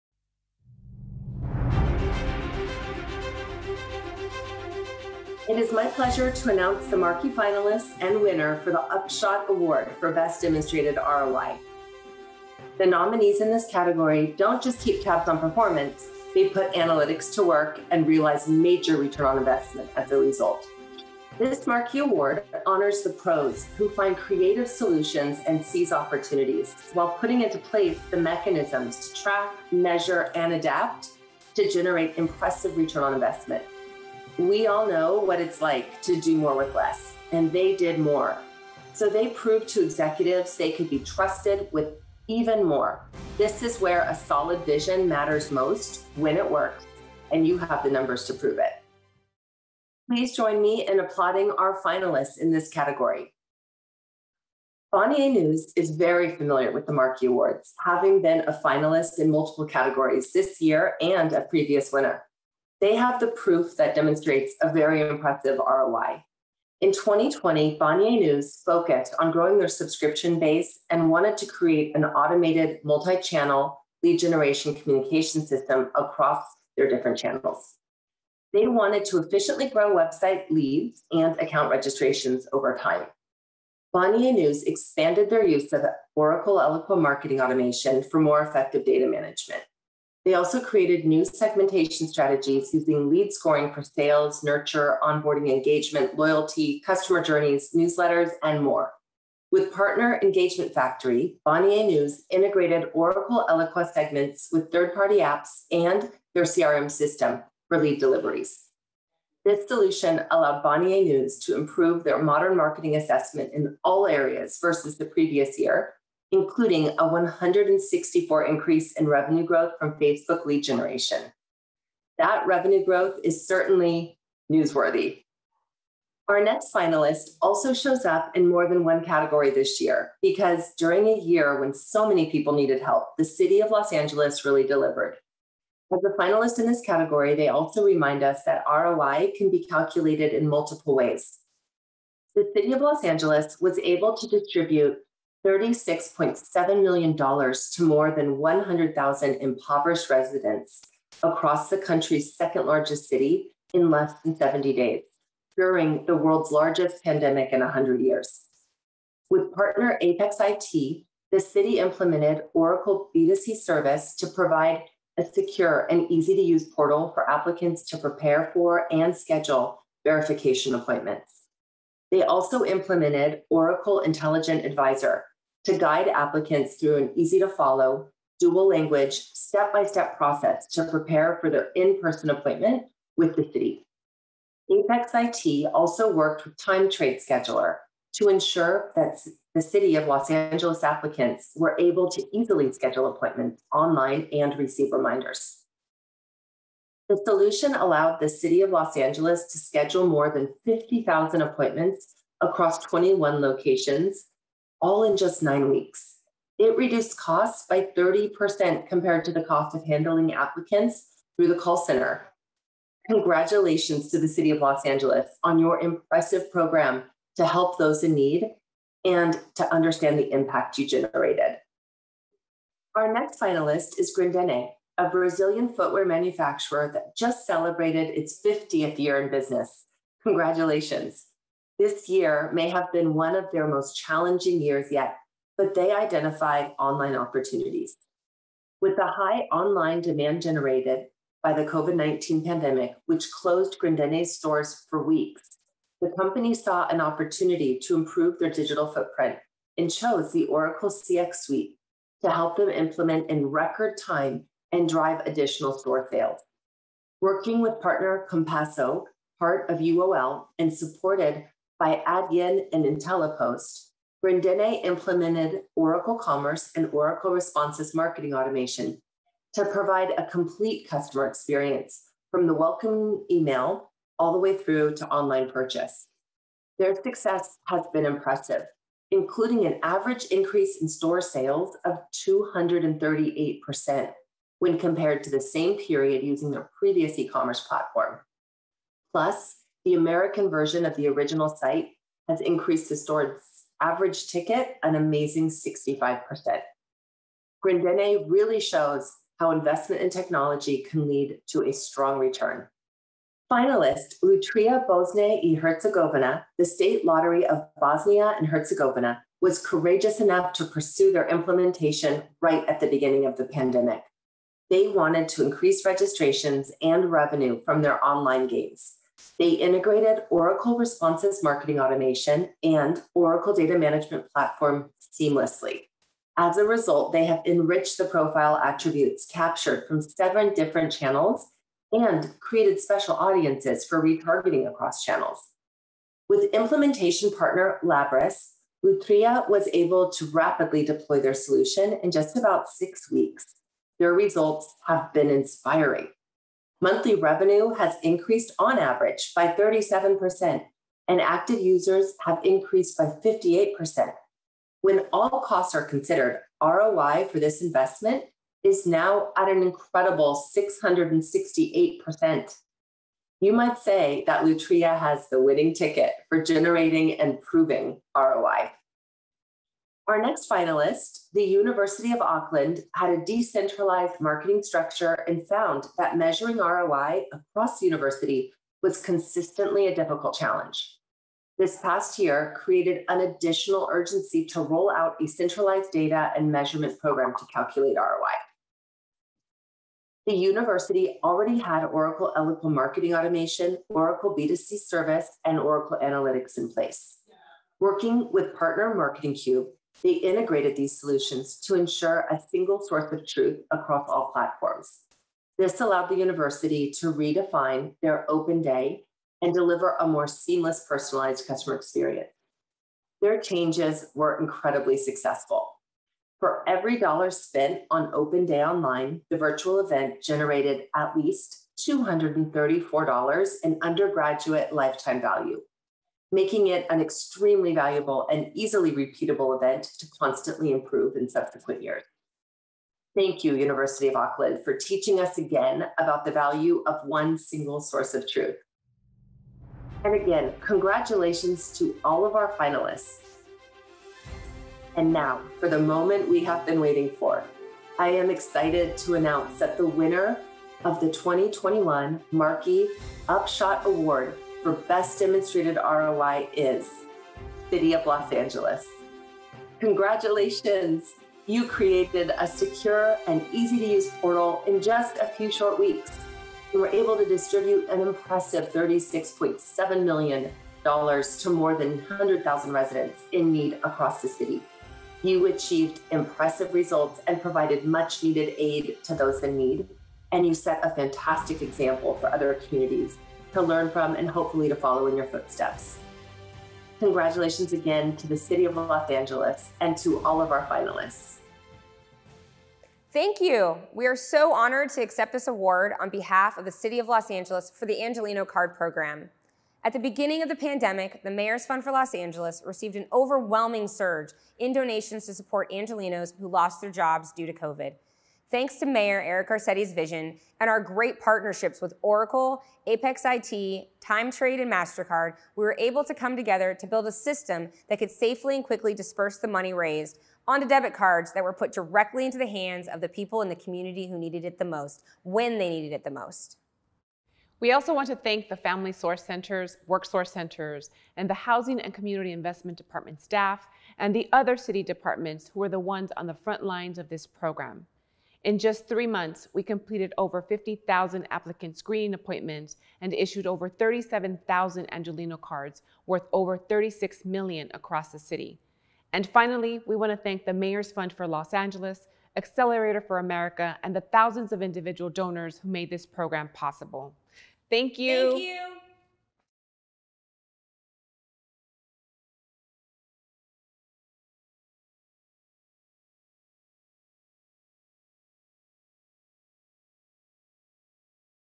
Markies—Upshot Award for Best Demonstrated ROI
Hear more about the 15th annual Markie Awards finalists and learn the winner of the Upshot Award for Best Demonstrated ROI.